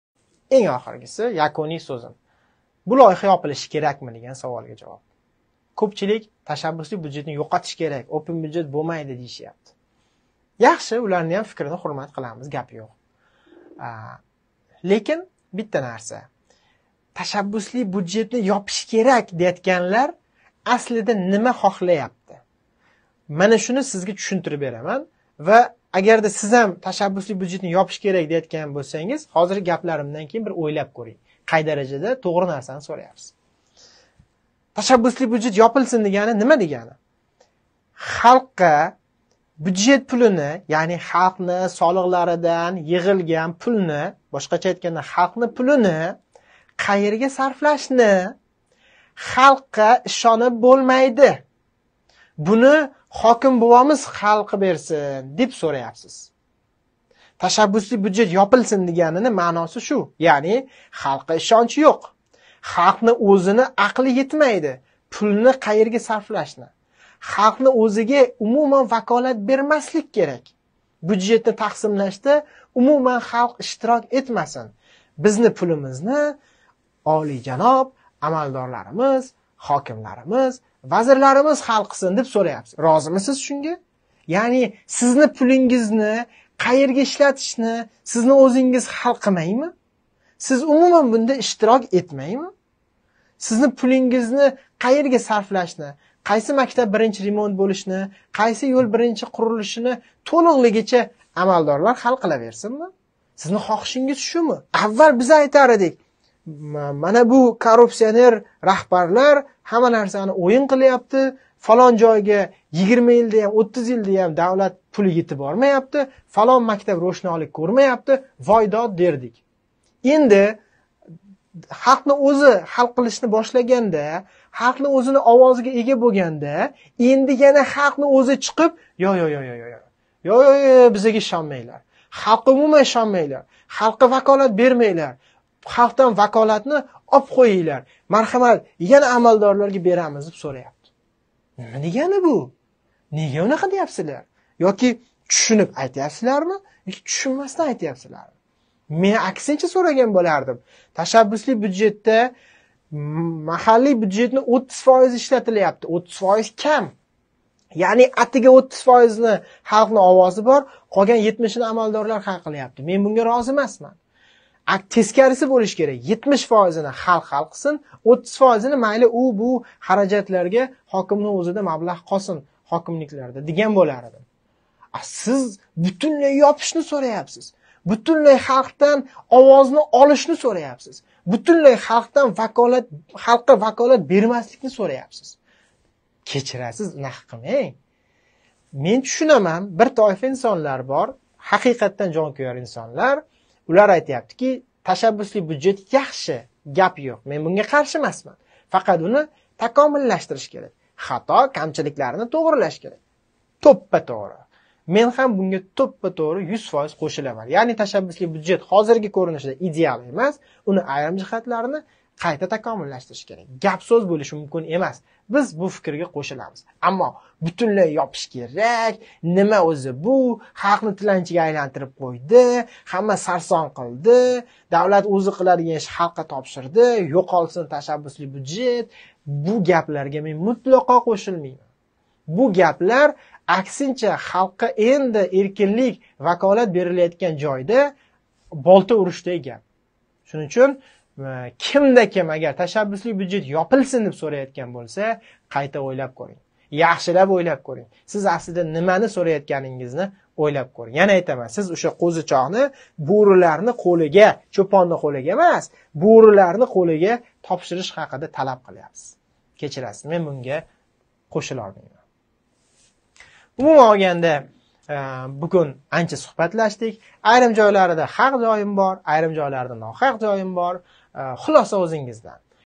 "Опен бюджет" ёпилсин, деб гапираётган ва бу лойиҳани устидан кулаётганлар видеони яхшилаб кўрсин.
"Опен бюджет" сабаб асфальт нималигини билмайдиган қишлоқ аҳли кўчаларига асфальт, сувсиз қийналаётган маҳаллаларга сувлар бормоқда. Ҳокимларнинг эсидан чиқиб кетган мактаблар таъмирланмоқда...

❗️Бу лойиҳани ёпилсин деб гапираётганларни маҳаллалари асфальт-да, уларга қизиқ эмас қишлоқ одамларининг кўчалари...